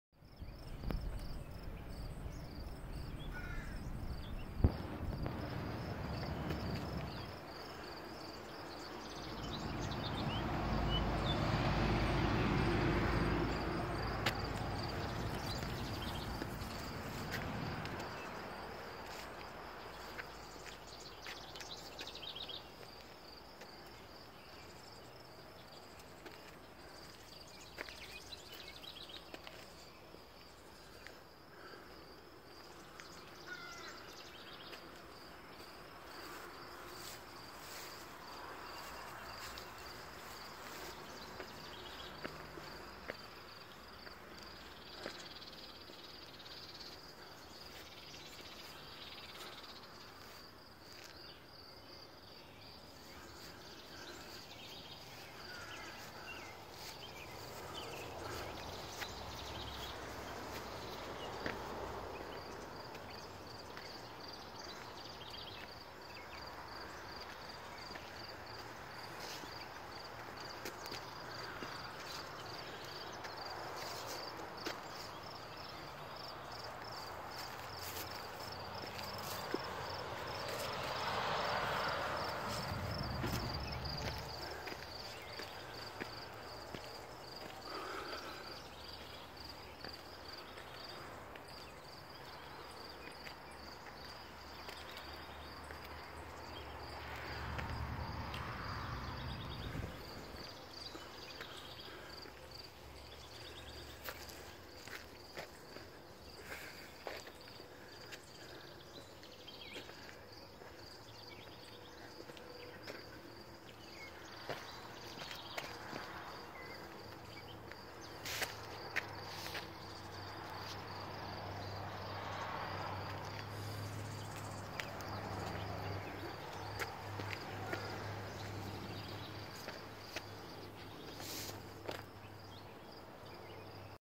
Janet's garden
This 34 year old Terrace Garden and the Mississippi River Valley tended by one very passionate